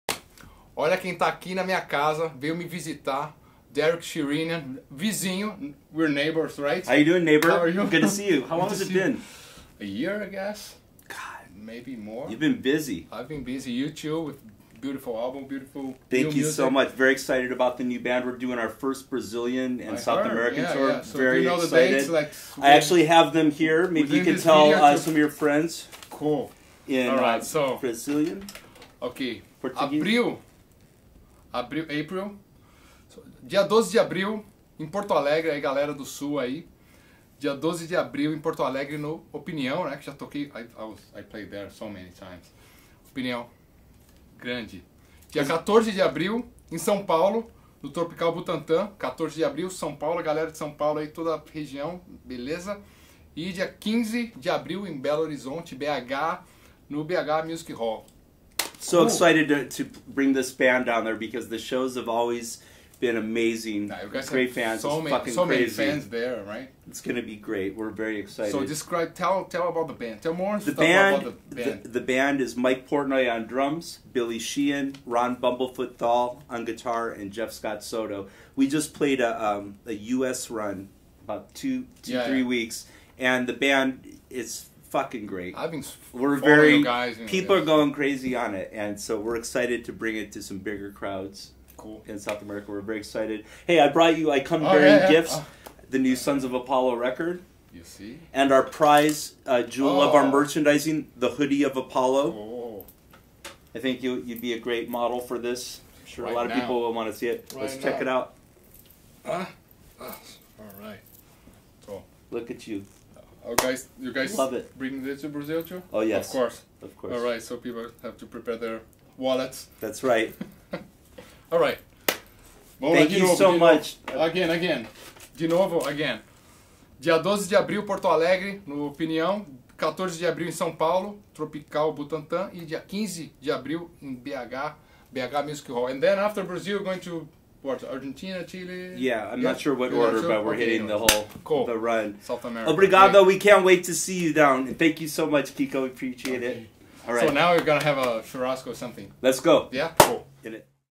Derek Sherinian and Kiko Loureiro
Discussing upcoming Sons Of Apollo tour dates